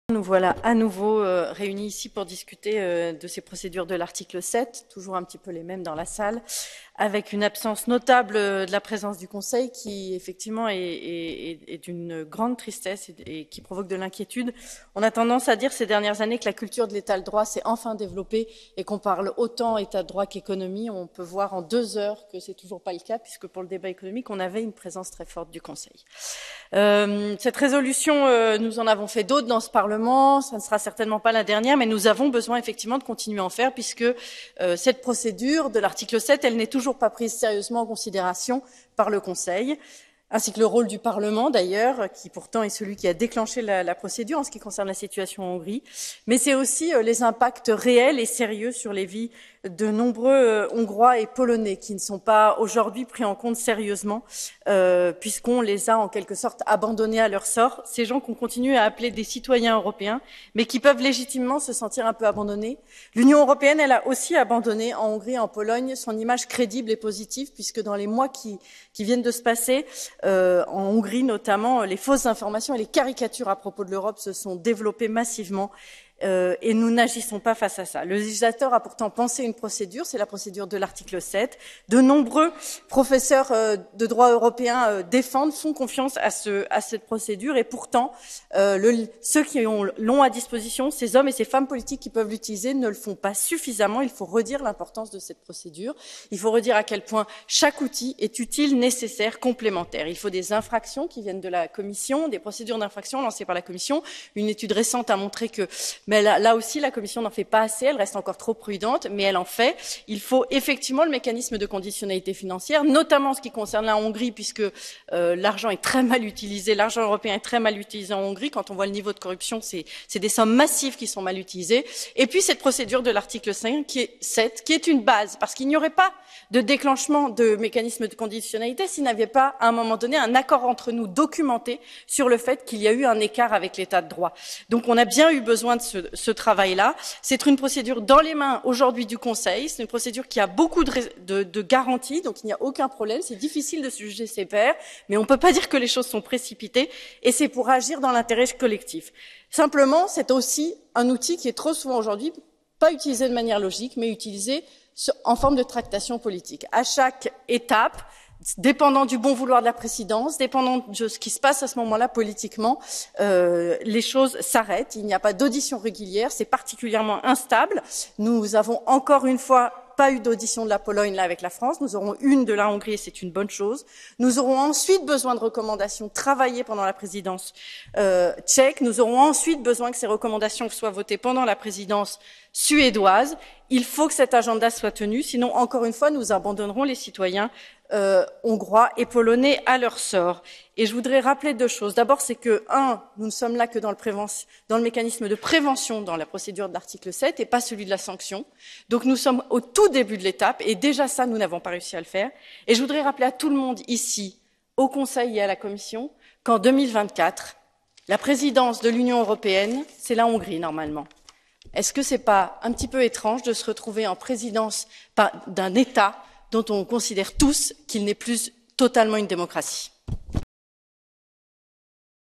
Gwendoline Delbos-Corfield sur les procédures Article 7 à l'encontre de la Pologne et de la Hongrie
Mardi 3 mai, Gwendoline Delbos-Corfield est intervenue sur les procédures de l'Article 7 en cours à l'encontre de la Pologne et de la Hongrie.

Son intervention ⬇️

"Nous voilà, à nouveau, réunis ici pour discuter de ces procédures de l'Article 7. Toujours un petit peu les mêmes dans la salle... Avec une absence notable de la présence du Conseil, qui effectivement est une grande tristesse et qui provoque de l'inquiétude. On a tendance à dire, ces dernières années, que la culture de l'État de droit s'est enfin développée et qu'on parle autant État de droit que d'économie. On peut voir en 2h que ce n'est toujours pas le cas puisque, pour le débat économique, on avait une présence très forte du Conseil. Cette résolution, nous en avons fait d'autres dans ce Parlement. Ça ne sera certainement pas la dernière, mais nous avons besoin effectivement de continuer à en faire, puisque cette procédure de l'Article 7 n'est toujours pas prise sérieusement en considération par le Conseil, ainsi que le rôle du Parlement, d'ailleurs, qui, pourtant, est celui qui a déclenché la procédure en ce qui concerne la situation en Hongrie. Mais c'est aussi les impacts réels et sérieux sur les vies de nombreux Hongrois et Polonais, qui ne sont pas, aujourd'hui, pris en compte sérieusement puisqu'on les a, en quelque sorte, abandonnés à leur sort.

Ces gens, qu'on continue à appeler des citoyens européens, mais qui peuvent légitimement se sentir un peu abandonnés. L'Union européenne, elle a, aussi, abandonné en Hongrie et en Pologne, son image crédible et positive puisque, dans les mois qui viennent de se passer, en Hongrie, notamment, les fausses informations et les caricatures à propos de l'Europe se sont développées massivement et nous n'agissons pas face à ça.

Le législateur a, pourtant, pensé une procédure, c'est la procédure de l'Article 7. De nombreux professeurs de droit européen défendent, font confiance, à cette procédure. Et, pourtant, ceux qui l'ont à disposition, ces hommes et ces femmes politiques qui peuvent l'utiliser, ne le font pas suffisamment. Il faut redire l'importance de cette procédure. Il faut redire à quel point chaque outil est utile, nécessaire, complémentaire.

Il faut des infractions qui viennent de la Commission, des procédures d'infraction lancées par la Commission. Une étude récente a montré que, là aussi, la Commission n'en fait pas assez. Elle reste encore trop prudente, mais elle en fait. Il faut, effectivement, le mécanisme de conditionnalité financière, notamment en ce qui concerne la Hongrie, puisque l'argent est très mal utilisé... L'argent européen est très mal utilisé en Hongrie, quand on voit le niveau de corruption, c'est des sommes massives qui sont mal utilisées. Et puis, cette procédure de l'Article 7, qui est une base parce qu'il n'y aurait pas de déclenchement de mécanisme de conditionnalité s'il n'y avait pas, à un moment donné, un accord entre nous, documenté, sur le fait qu'il y a eu un écart avec l'État de droit.

Donc on a bien eu besoin de ce travail-là. C'est une procédure dans les mains, aujourd'hui, du Conseil. C'est une procédure qui a beaucoup de garanties, donc il n'y a aucun problème. C'est difficile de juger ses pairs, mais on ne peut pas dire que les choses se sont précipitées et c'est pour agir dans l'intérêt collectif. Simplement, c'est aussi un outil qui est trop souvent, aujourd'hui, pas utilisé de manière logique mais utilisé en forme de tractations politiques. À chaque étape, dépendant du bon vouloir de la Présidence, dépendant de ce qu'il se passe à ce moment-là politiquement, les choses s'arrêtent, il n'y a pas d'audition régulière, c'est particulièrement instable. Nous n'avons, encore une fois, pas eu d'audition de la Pologne, là, avec la France. Nous en aurons une de la Hongrie et c'est une bonne chose. Nous aurons ensuite besoin de recommandations, travaillées pendant la présidence tchèque. Nous aurons ensuite besoin que ces recommandations soient votées pendant la présidence suédoise.

Il faut que cet agenda soit tenu. Sinon, encore une fois, nous abandonnerons les citoyens hongrois et polonais à leur sort. Et je voudrais rappeler deux choses. D'abord, c'est que nous ne sommes là que dans le mécanisme de prévention, dans la procédure de l'Article 7, et pas celui de la sanction. Donc, nous sommes au tout début de l'étape et, déjà ça, nous n'avons pas réussi à le faire.

Et je voudrais rappeler à tout le monde, ici, au Conseil et à la Commission, qu'en 2024, la présidence de l'Union européenne, c'est la Hongrie, normalement. Est-ce que ce n'est pas un petit peu étrange de se retrouver en présidence d'un État dont on considère tous qu'il n'est plus totalement une démocratie ?"